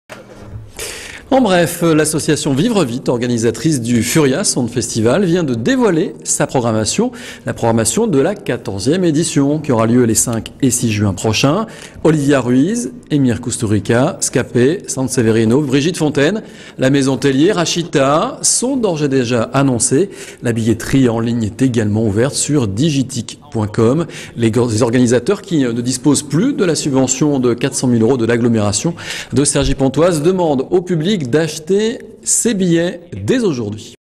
Furia 2010 : la programmation !
Au programme, entre autres, de 14ème édition du Furia 2010 : Olivia Ruiz, Emir Kusturica, Ska-P, Brigitte Fontaine... (DROITS RESERVES . Pour toute exploitation sur TV, web, mobile, aller